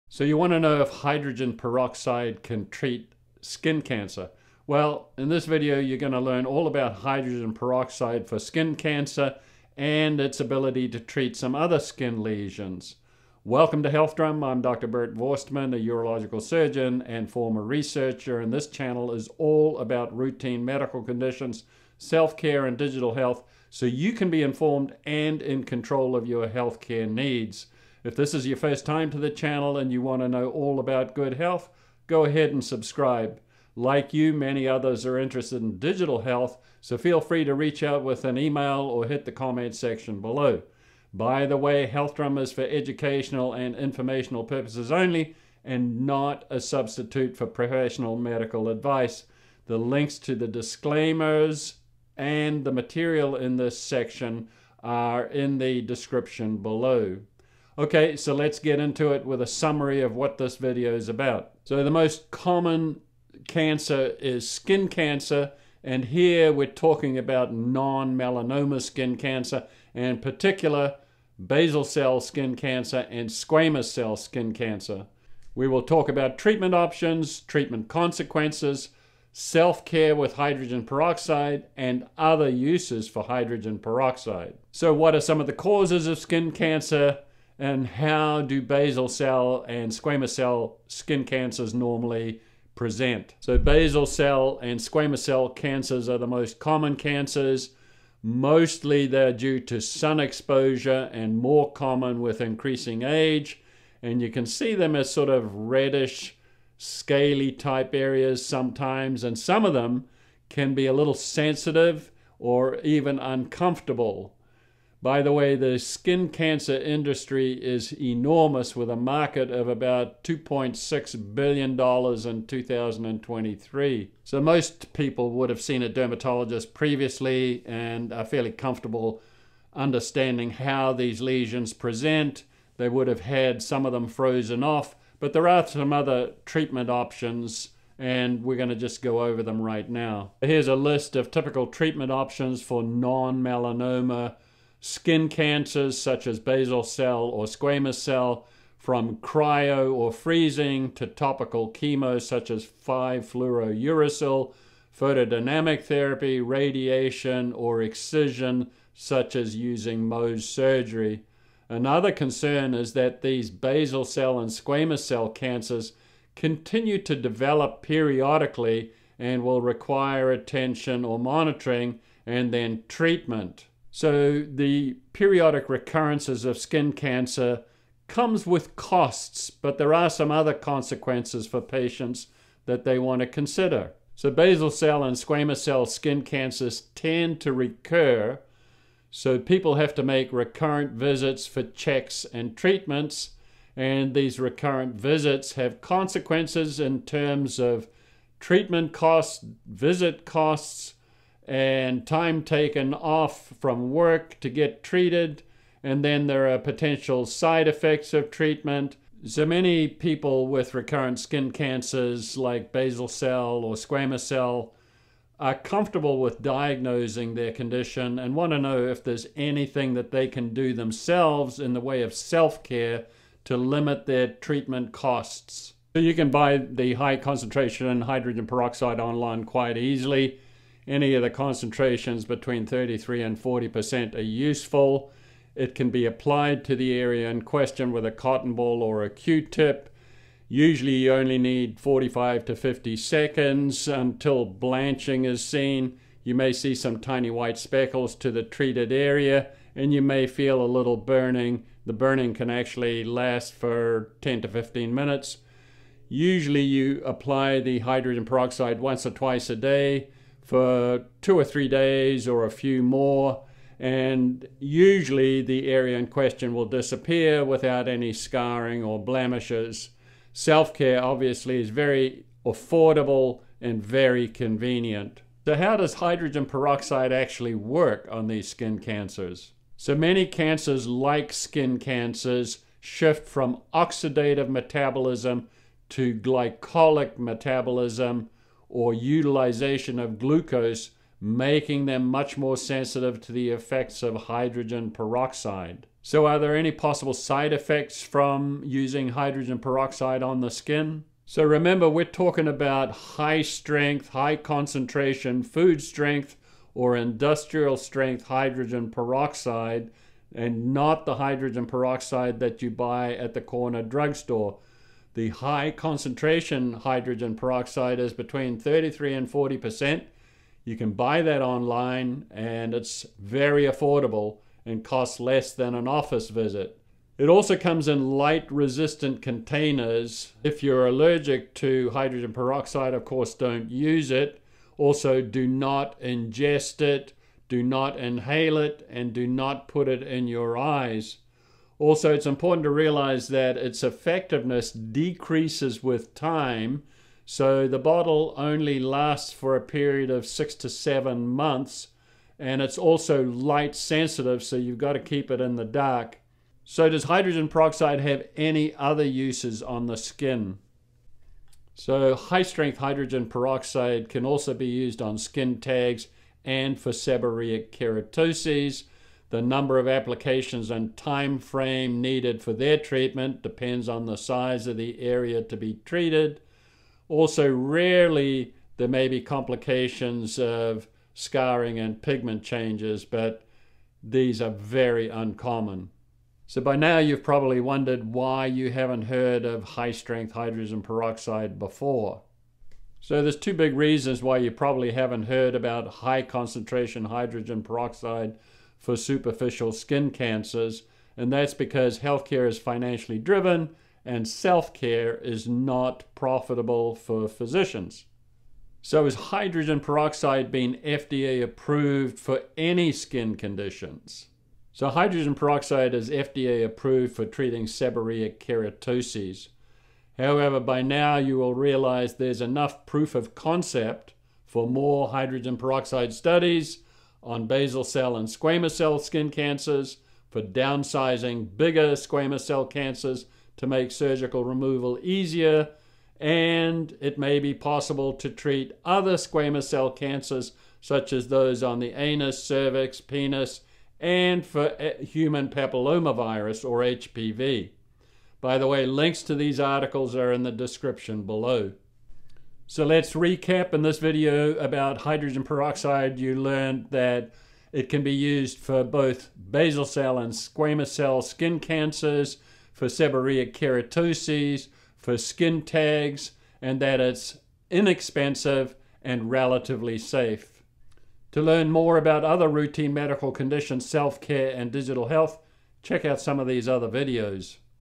How does hydrogen peroxide treat skin cancer? : localized skin cancer
How does hydrogen peroxide treat skin cancer? How can hydrogen peroxide treat localized skin cancer?
In this video you will learn that:
- skin cancer is extremely common
-  most are either basal cell or squamous cell skin cancers
- most are superficial, they're due to sun exposure, more common with advancing age
- most are easily treated with topical medications or industrial strength (33-40%) hydrogen peroxide
- self care treatments are convenient and affordable

Links.
Hydrogen peroxide.